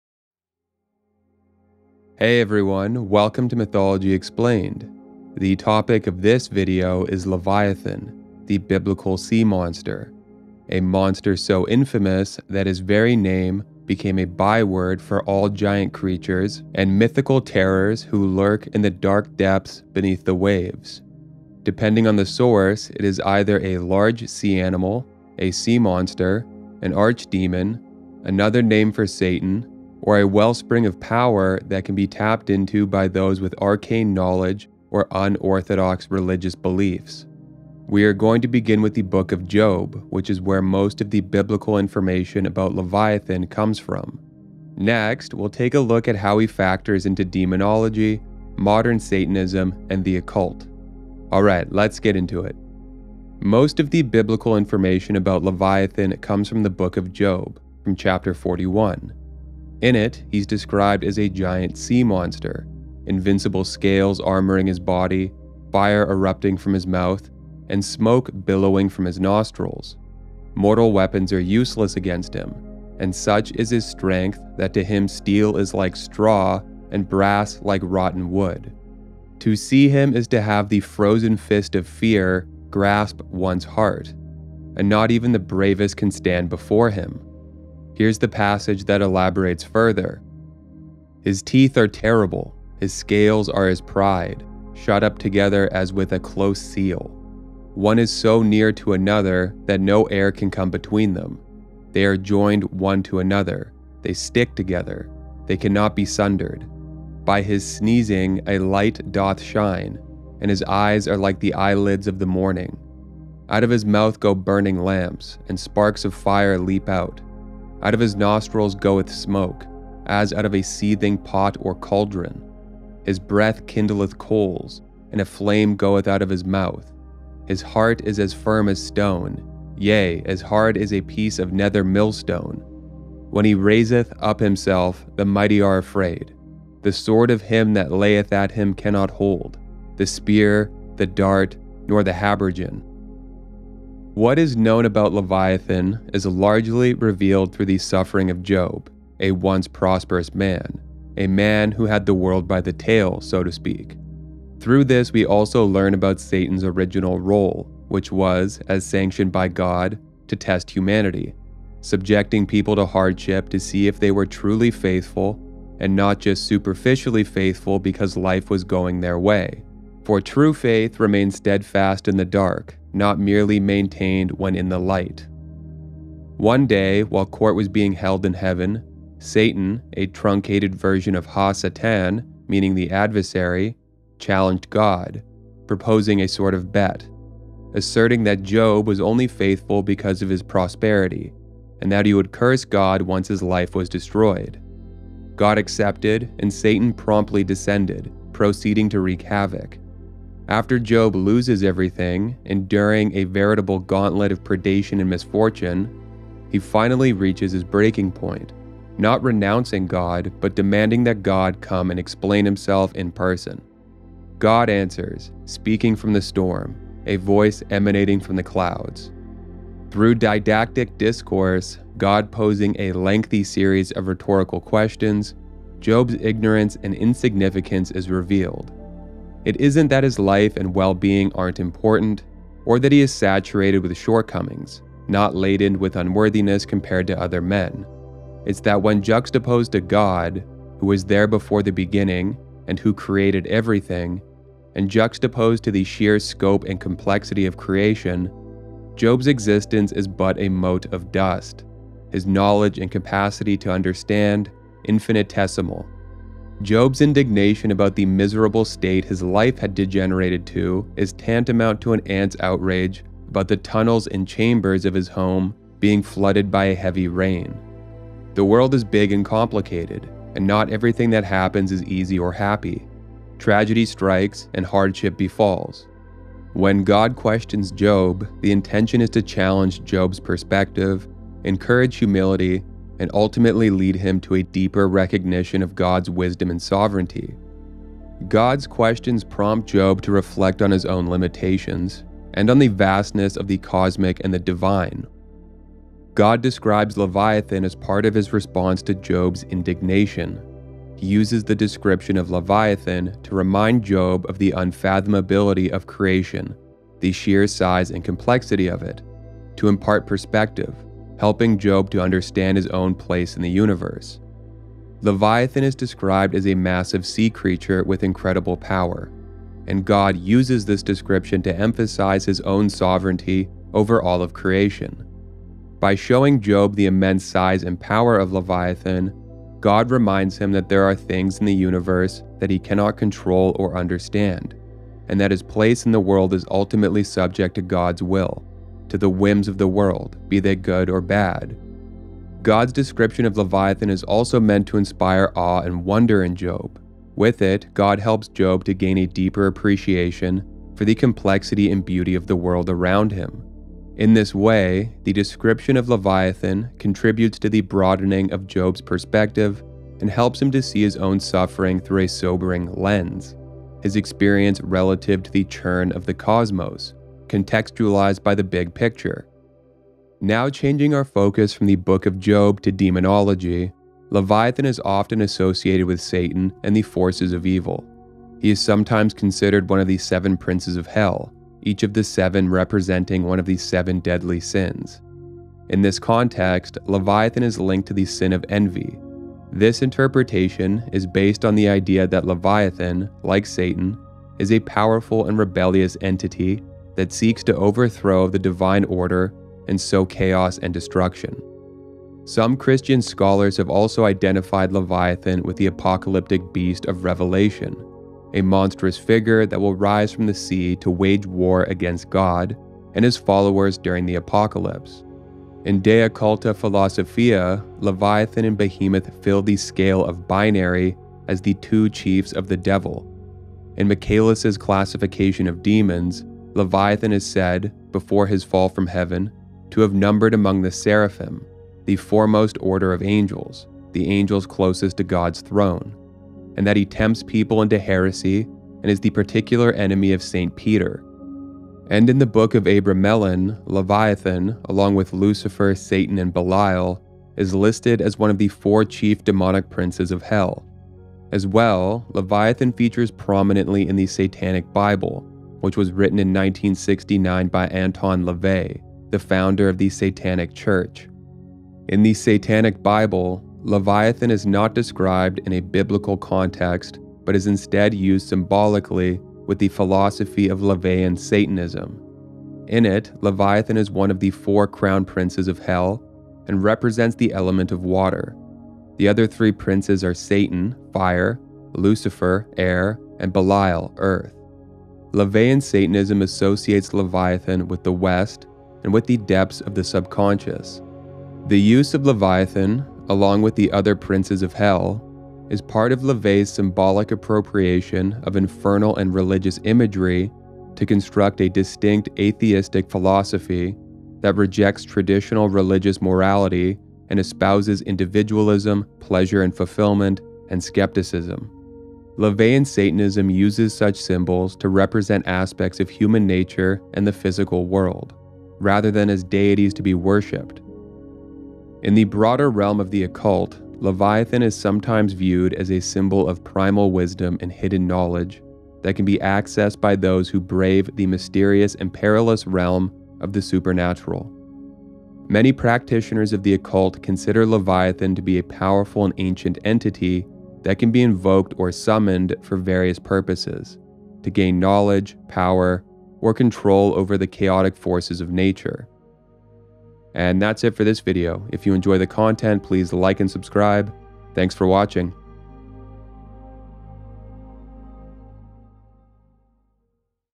Leviathan: One of the 7 Deadly Demons & the Terror of the Deep
Hey everyone, welcome to Mythology Explained. The topic of this video is Leviathan, the biblical sea monster, a monster so infamous that his very name has become a byword for all giant creatures and mythical terrors who lurk in the dark depths beneath the waves. Depending on the source, it is either a large sea animal, a sea monster, an archdemon, another name for Satan, or a wellspring of power that can be tapped into by those with arcane knowledge or unorthodox religious beliefs.

We’re going to begin with the Book of Job, which is where most of the biblical information about Leviathan comes from. Next, we’ll take a look at how he factors into demonology, modern Satanism, and the occult.

Alright, let’s get into it.

Most of the Biblical information about Leviathan comes from the Book of Job, from chapter 41. In it, he’s described as a giant sea monster: invincible scales armoring his body, fire erupting from his mouth, and smoke billowing from his nostrils. Mortal weapons are useless against him, and such is his strength that to him steel is like straw and brass like rotten wood. To see him is to have the frozen fist of fear grasp one’s heart, and not even the bravest can stand before him.

 Here’s the passage that elaborates further:

 “His teeth are terrible… His scales are his pride, shut up together as with a close seal. One is so near to another, that no air can come between them. They are joined one to another, they stick together, that they cannot be sundered. By his… [sneezing] a light doth shine, and his eyes are like the eyelids of the morning. Out of his mouth go burning lamps, and sparks of fire leap out. Out of his nostrils goeth smoke, as out of a seething pot or cauldron. His breath kindleth coals, and a flame goeth out of his mouth…his heart is as firm as stone; yea, as hard as a piece of the nether millstone. When he raiseth up himself, the mighty are afraid…The sword of him that layeth at him cannot hold: the spear, the dart, nor the habergeon.”

Watch the video for the rest!